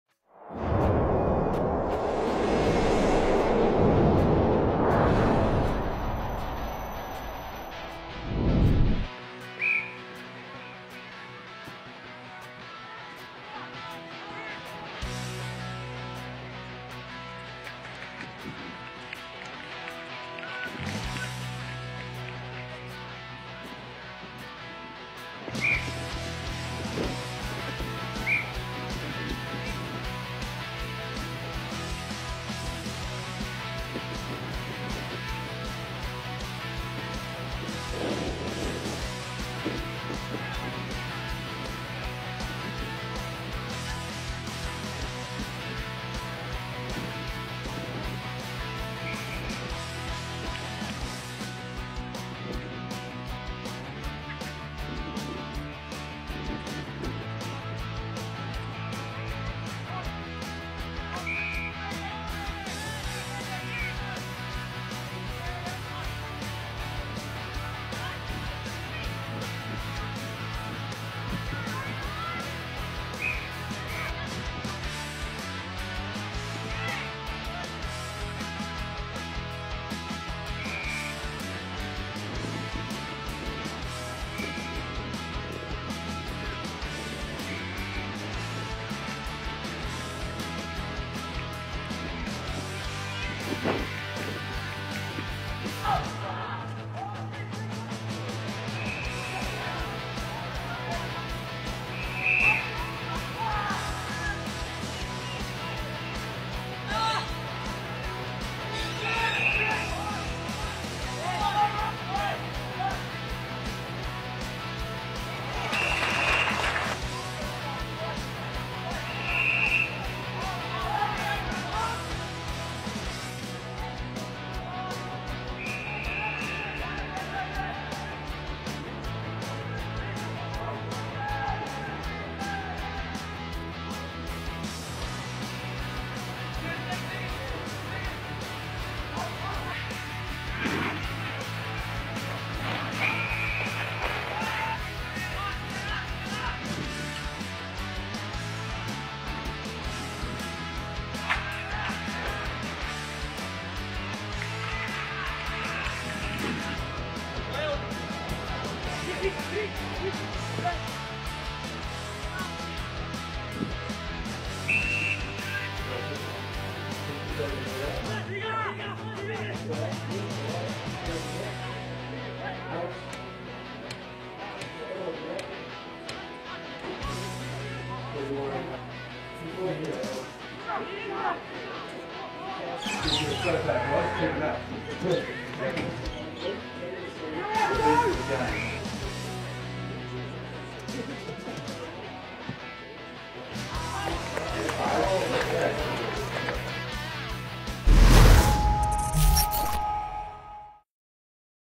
Pick 62: Matthew Signorello
Check out the highlights of the Crows latest draftee, Pick 62 - Matthew Signorello - Midfielder from Northern Knights (Vic)